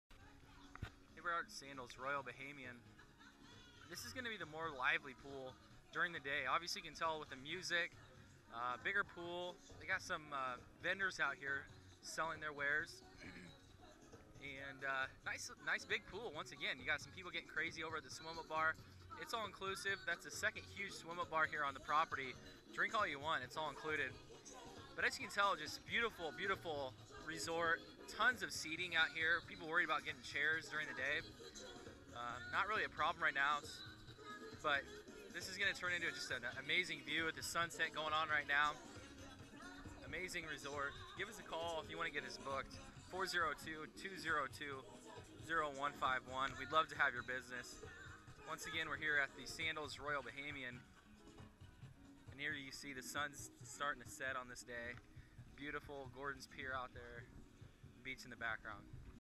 Sandals Royal Bahamian Loud Pool
Tour of the Sandals Royal Bahamian Loud Pool provided by Reinke Travel Company. Give us a call we would love to have your business.

Matt Reinke
Reinke Travel Company